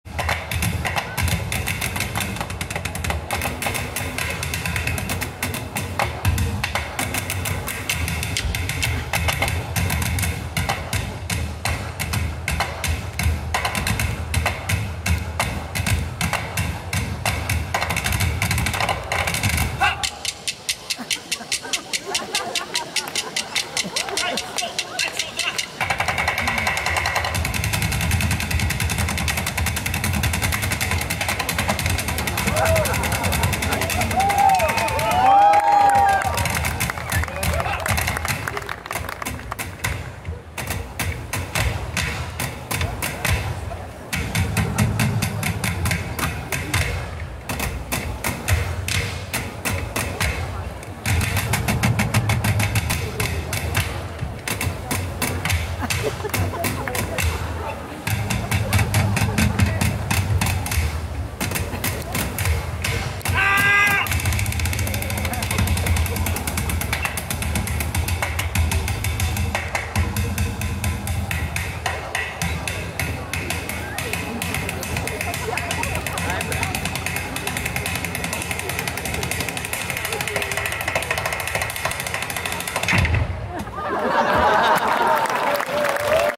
Elbtonal Percussions in Hochform - Quellendankfeier 2015 Bad Nauheim
Ausschnitt aus den Darbietungen der Elbtonal Percussions anlässlich der Quellendankfeier im Sprudelhof Bad Nauheim 2015